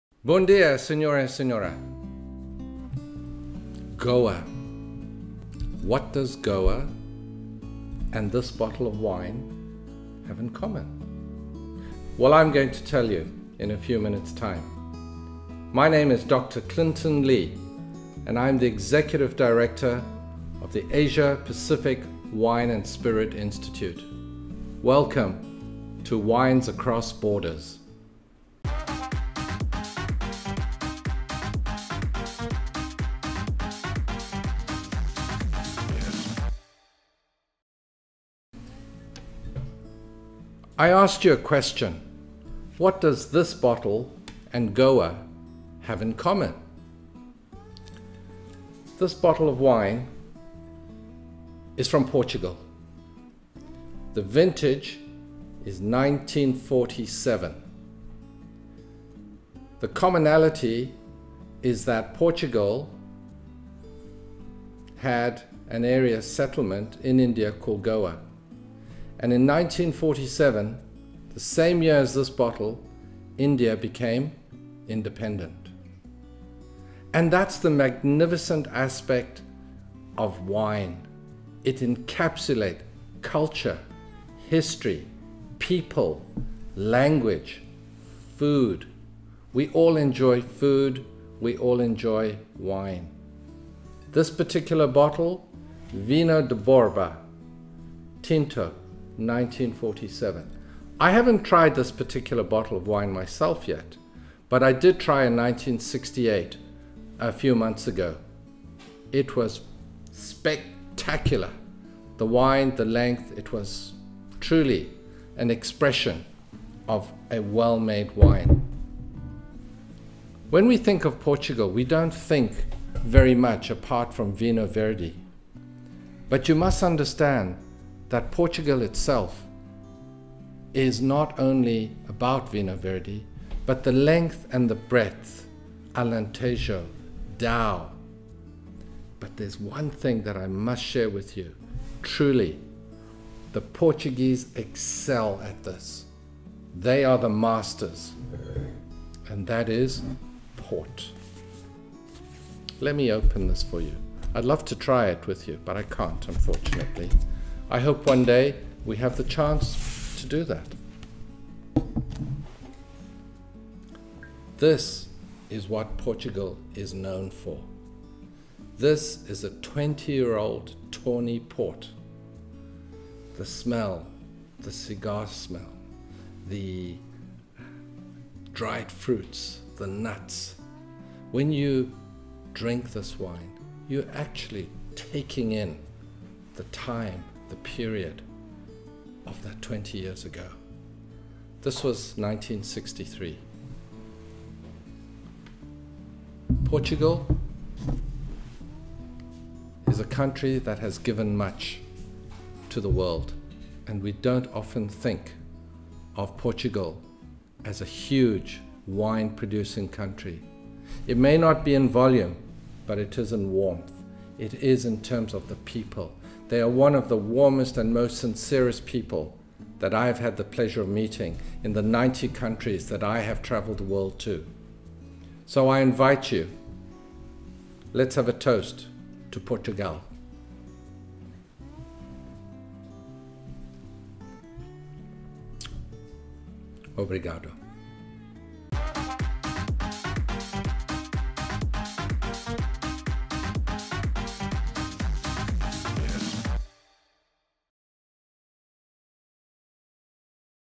Bem Vinda - Portugal
Welcome to our "Wines Across Borders Series" with Dr. Clinton Lee. This time Dr. Lee talks Portuguese wines.